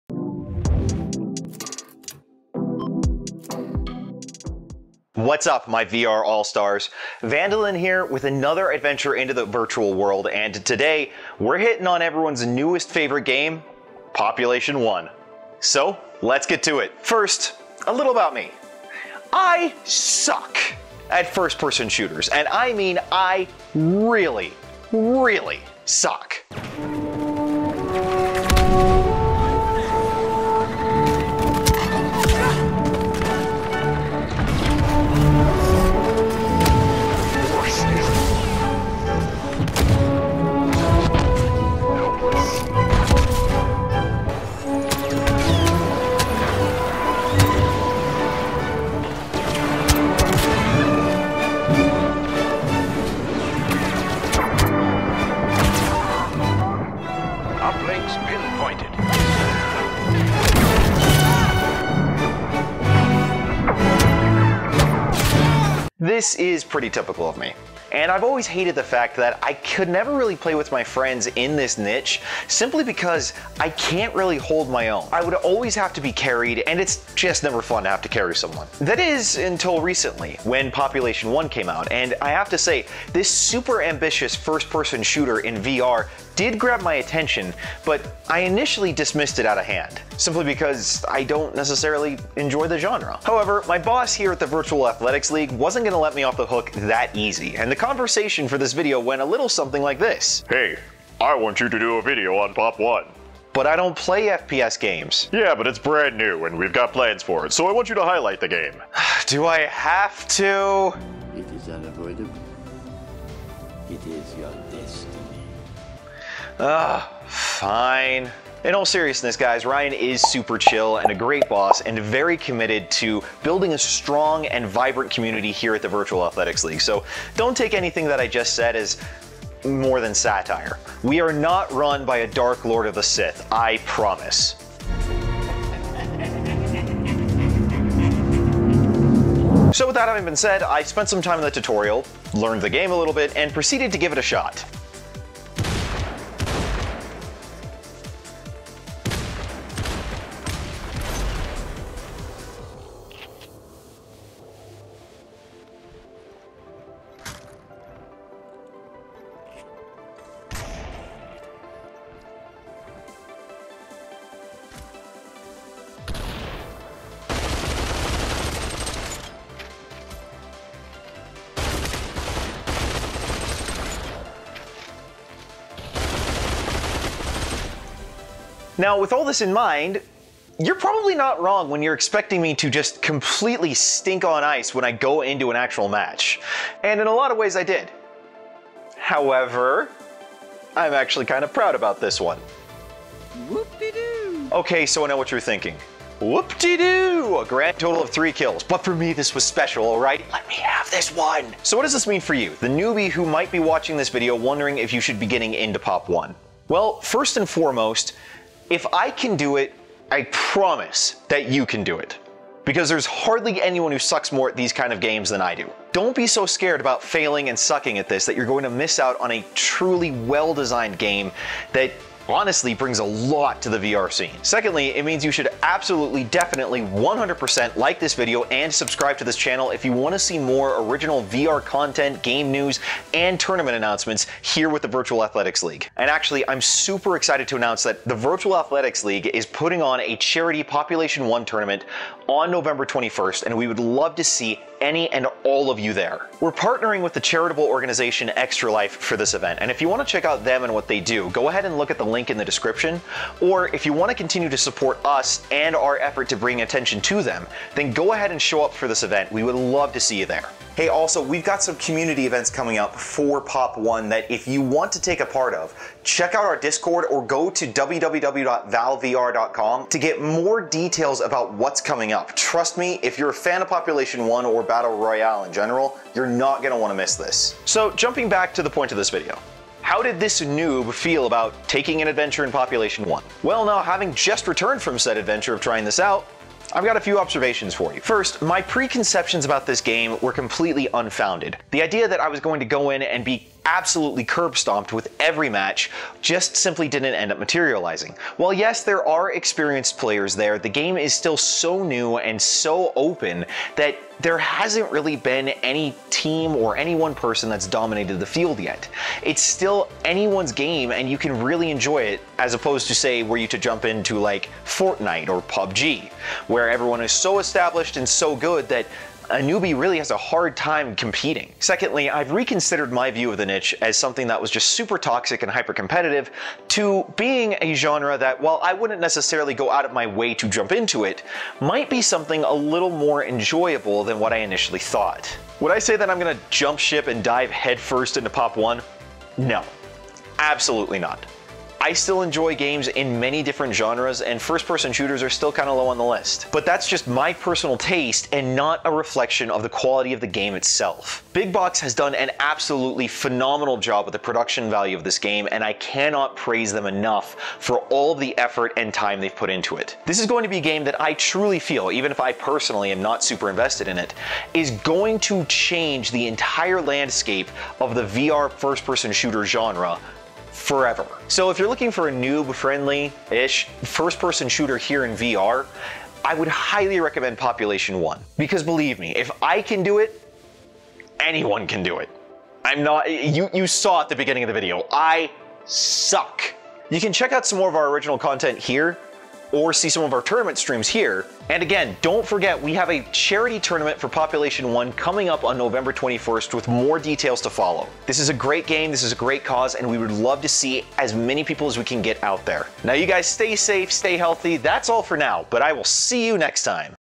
Is Population: One noob friendly? An honest review of VR's hottest new game on Quest 2!
Have you looked at Population: One and thought it might be a game you'd enjoy, but you worry that you'll be steamrolled by a million and one high level players with insane skills you can only dream of? Well here's an honest review of the game everyone is talking about.